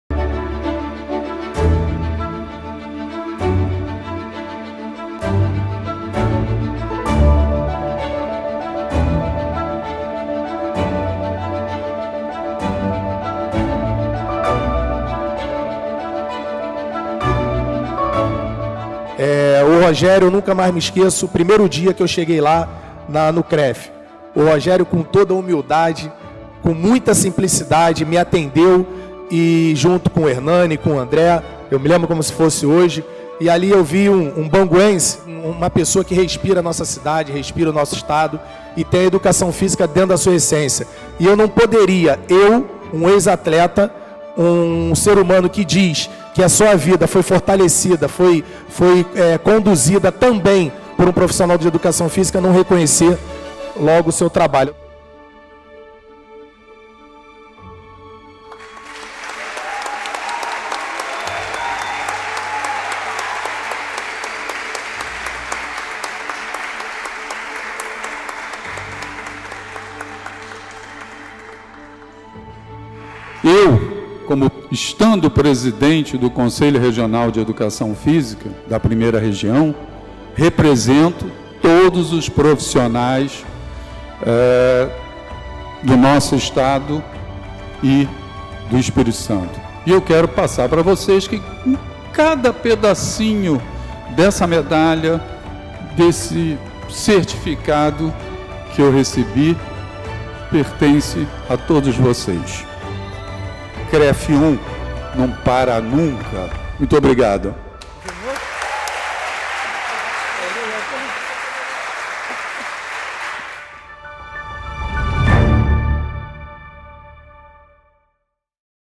Presidente do CREF1, prof. Rogério Melo é homenageado com Medalha Pedro Ernesto
Presidente do CREF1, prof. Rogério Melo, é congratulado com a Medalha de Mérito Pedro Ernesto em solenidade em comemoração ao Dia do Profissional de Educação Física, promovida no dia 30 de agosto, na Câmara Municipal do Rio de Janeiro. No evento, outros seis profissionais foram congratulados com a moção de aplausos da casa e também receberam a Medalha da Frente Parlamentar e o diploma Honorário do Conselho.